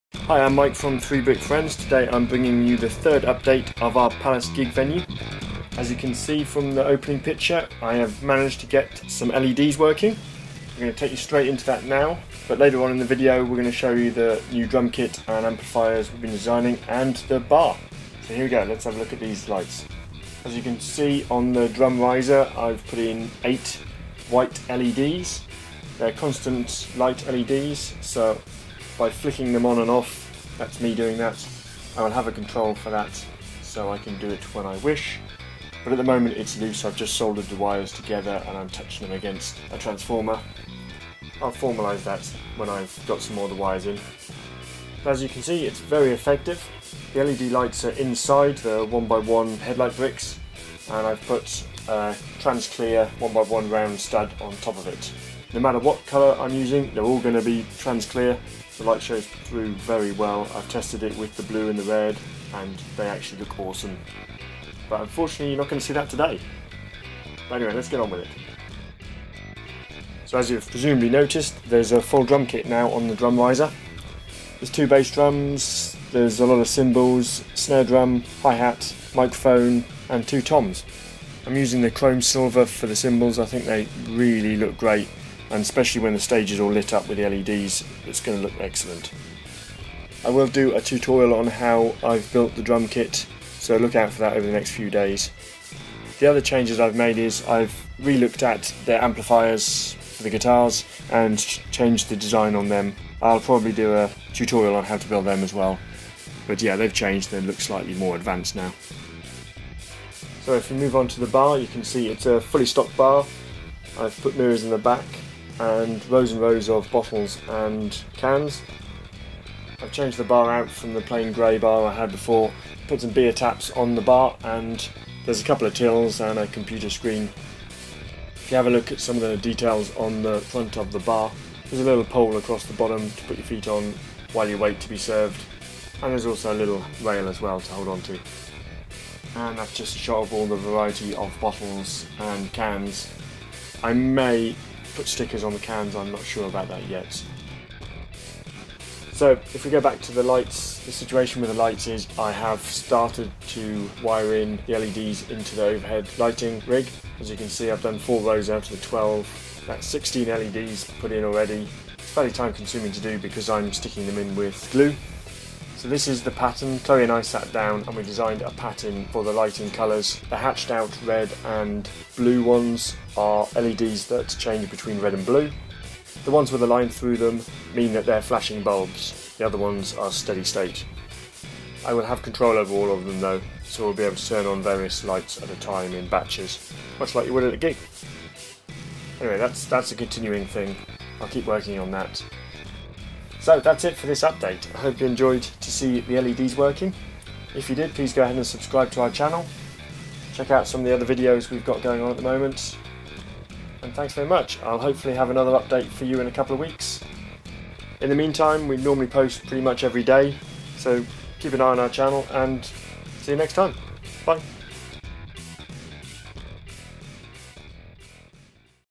Lego Palace Gig Venue - Update 3 - Drums & Lights
This third update of our Lego Palace Gig Venue MOC shows the progress made in developing the LED lighting and drum kit.

The Lego creation by 3BrickFriends is based on the Lego Creator Palace Cinema modular building (lego 10232).

We publish a lot of videos showing our Lego MOCs, haul purchases, blogs and tutorials.